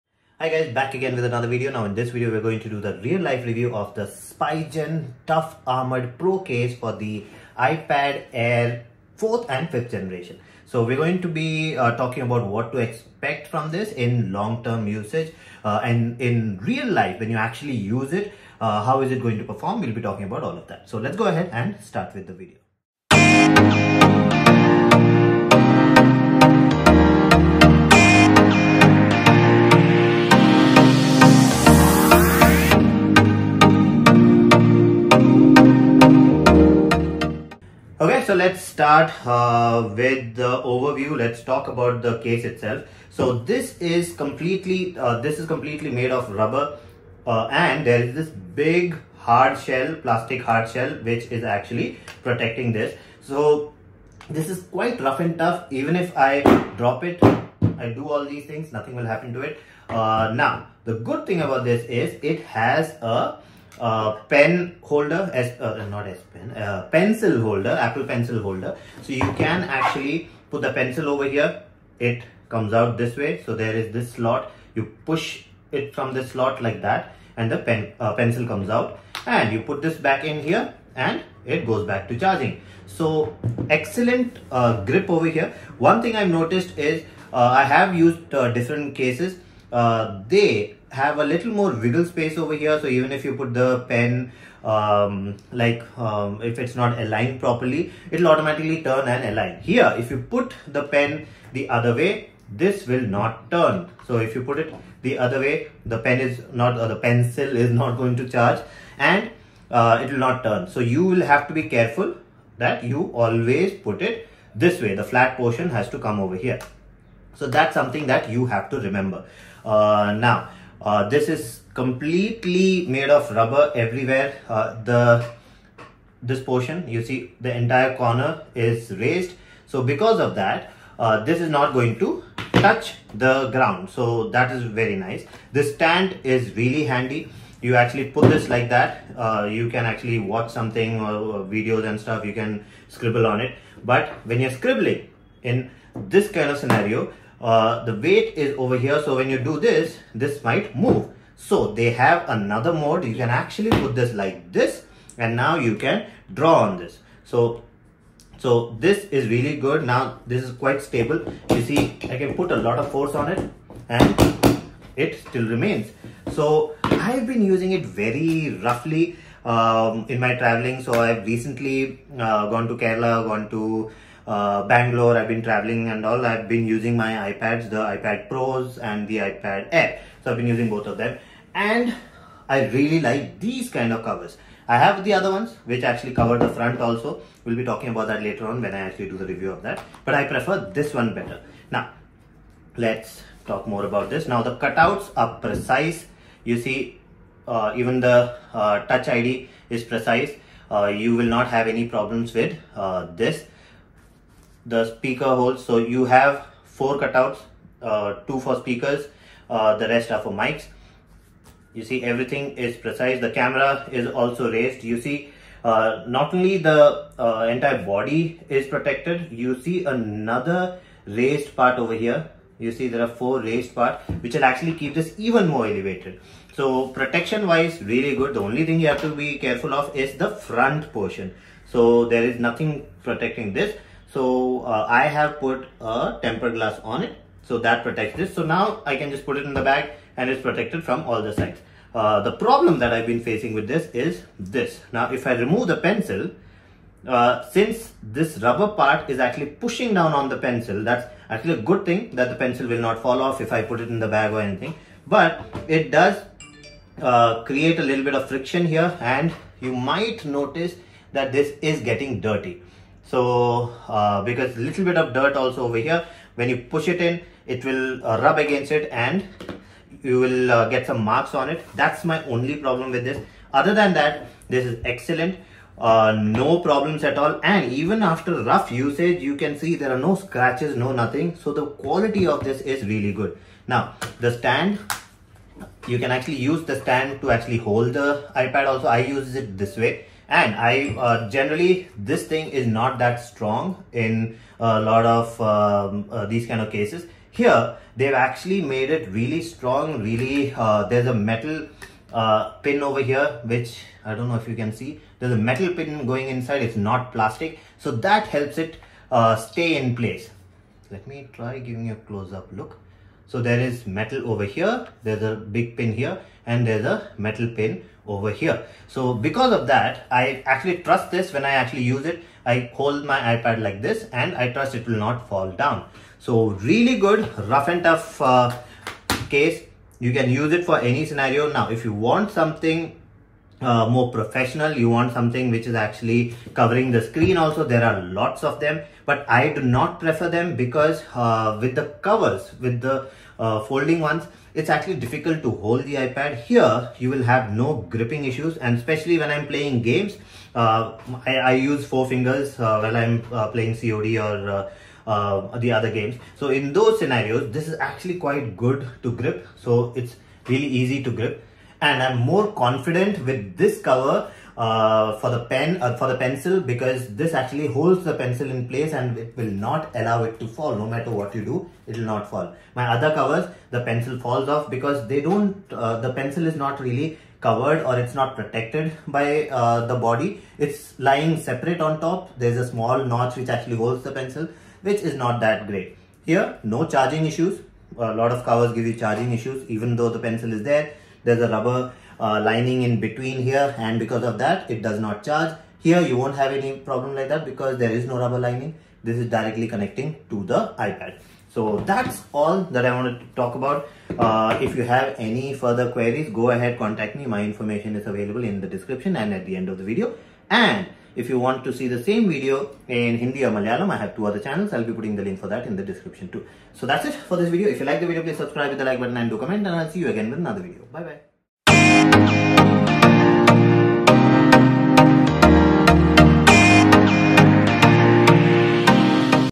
Spigen Tough Armor Pro Case for iPad Air 10.9 inch 4th & 5th Gen - (Real Life Review) (English)
In this video we will be doing the Real Life Review of the Spigen Tough Armor Pro Case for  iPad Air 10.9 inch 4th & 5th Gen and talk about what to expect from it.

Purchase Link:
Spigen Tough Armor Pro Case for  iPad Air 10.9 inch 4th & 5th Gen (Black)